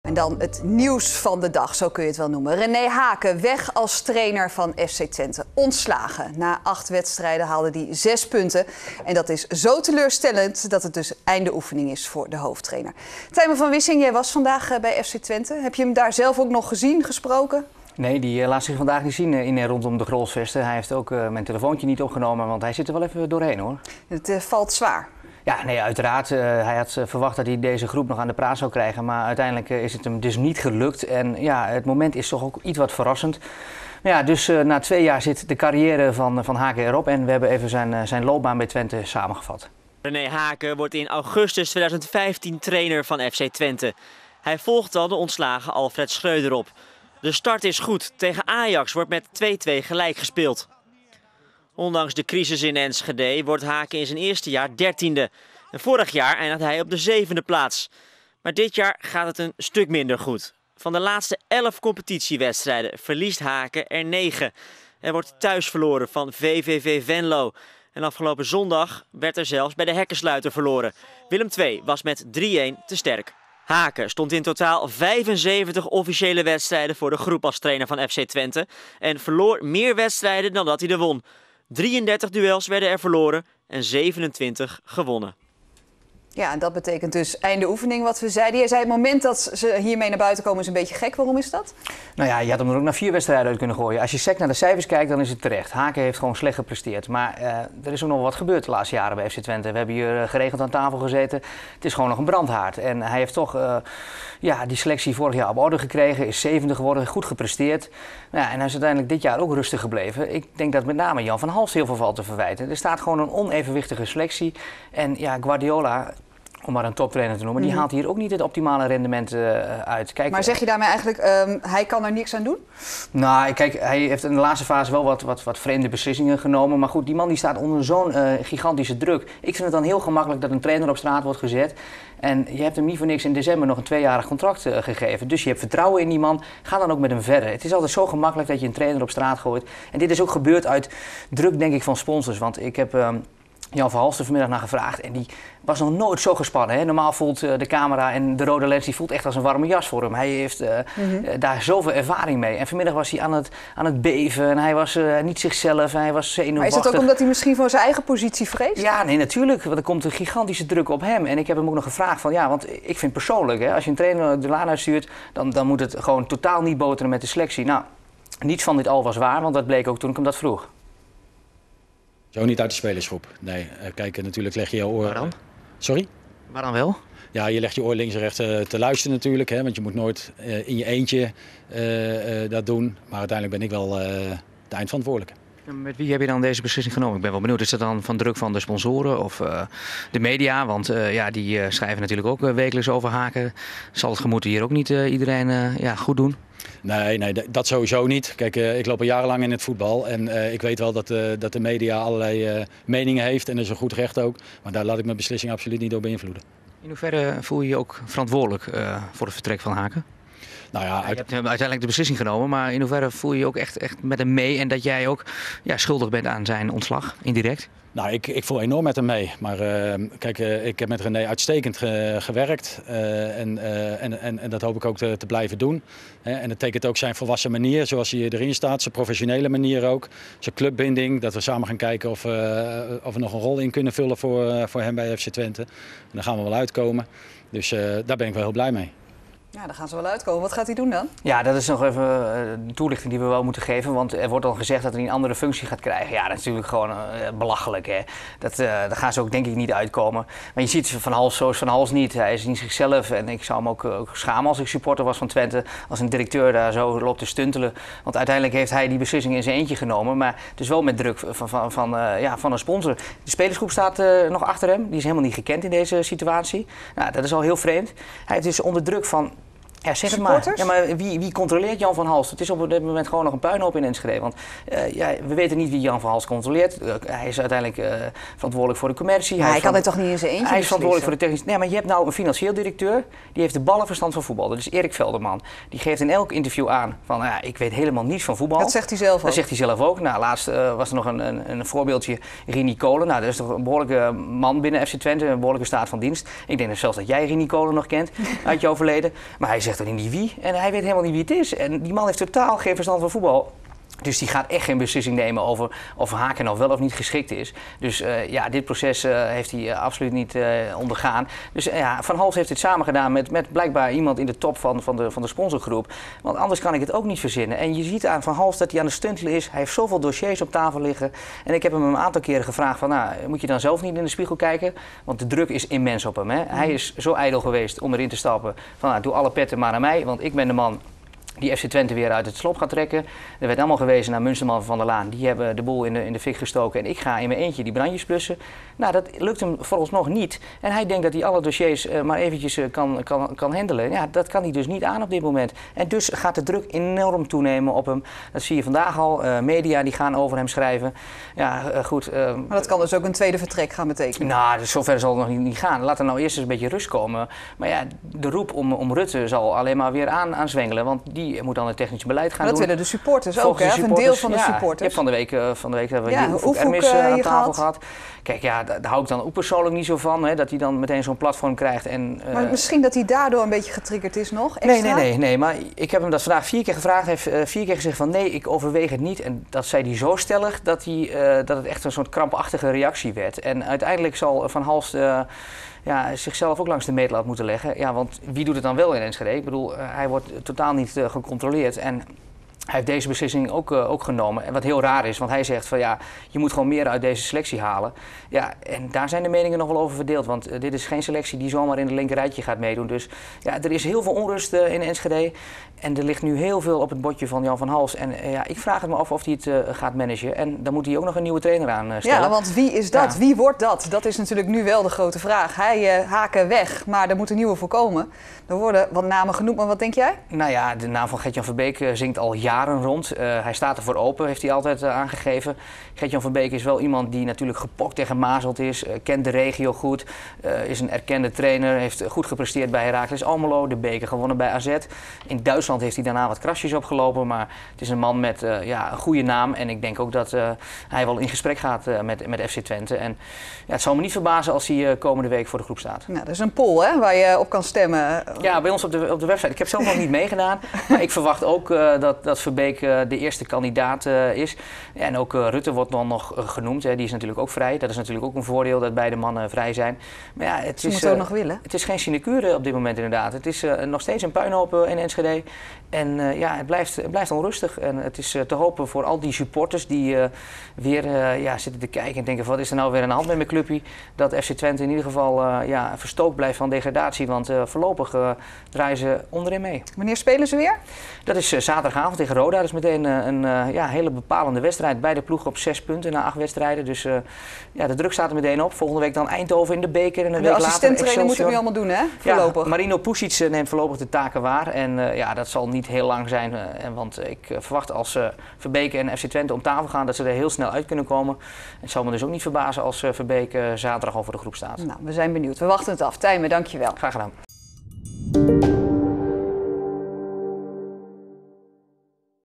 René Hake weg als trainer bij FC Twente
Ren Hake is niet langer de trainer van FC Twente. Assistent-trainer Marino Pusic neemt zijn taken voorlopig over.